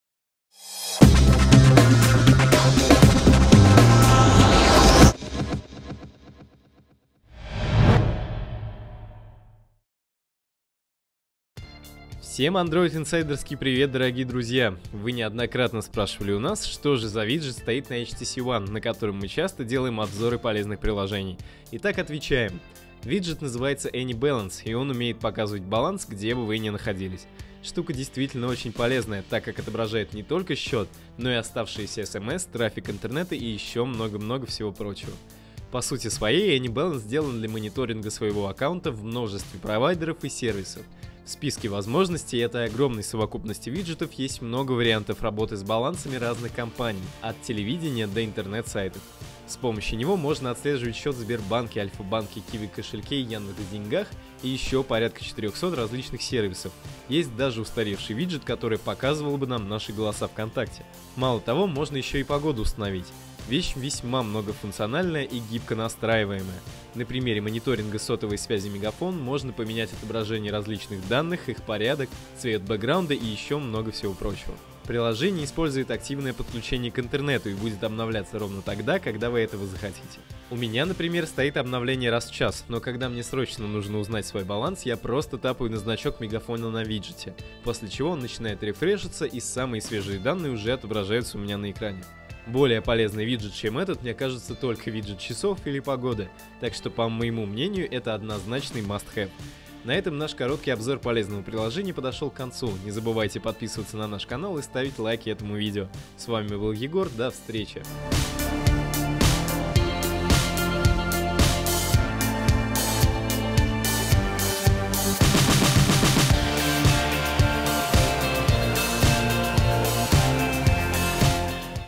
Как отобразить баланс на смартфоне с ОС Android?
Отличный виджет для тех, кому важено знать, сколько средств осталось на вашем счету мобильного оператора, и не только! Помимо этого возможен мониторинг банковских счетов, электронных денег и прочего.